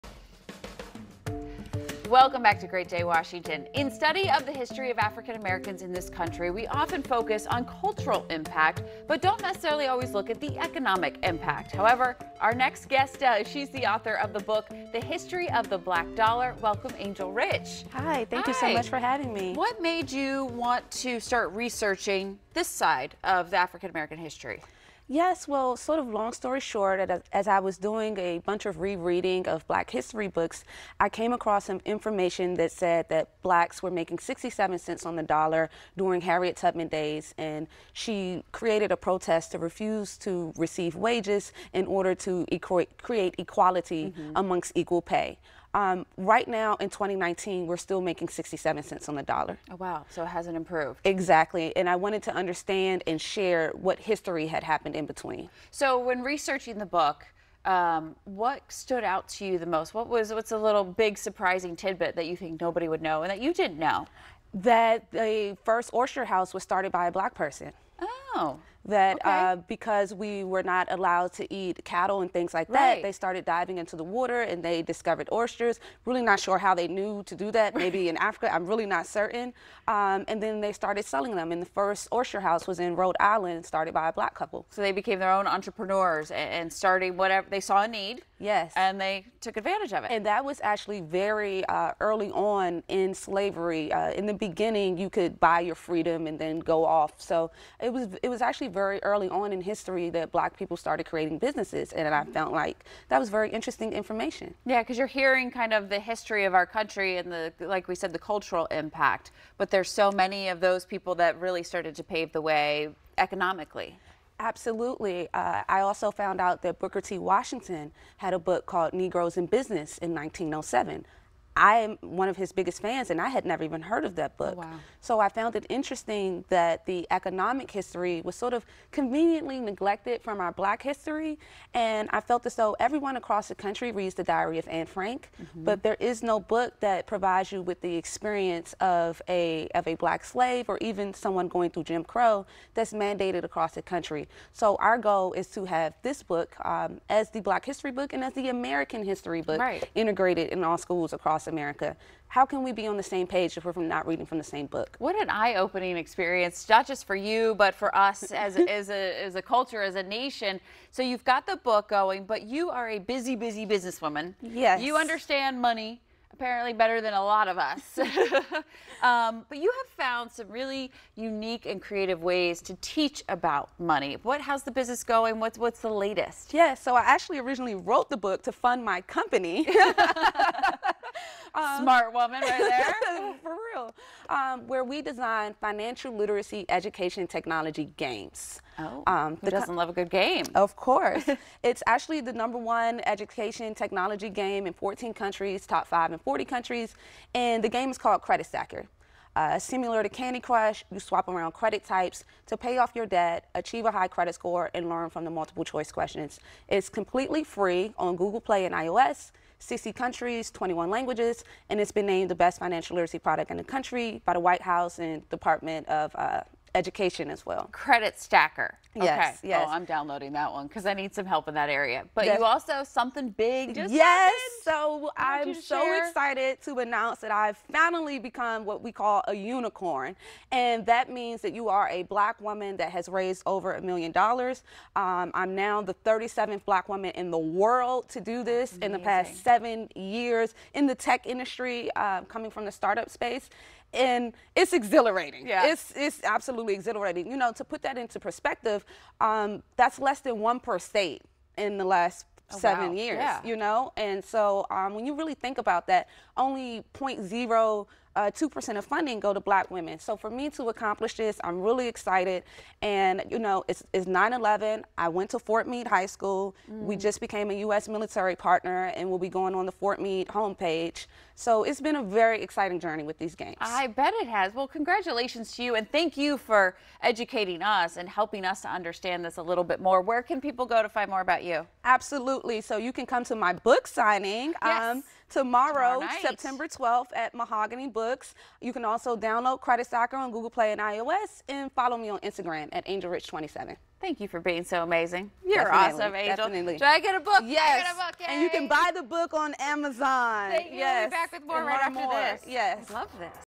Author Angel Rich seeks to educate the public on financial literacy in new book
Author Angel Rich talks about her latest book, History of the Black Dollar, which highlights the economic impact African Americans have had in US history. To meet Rich, she will be hosting a book signing September 12, 2019 from 5-7 p.m. at Mahogany Books.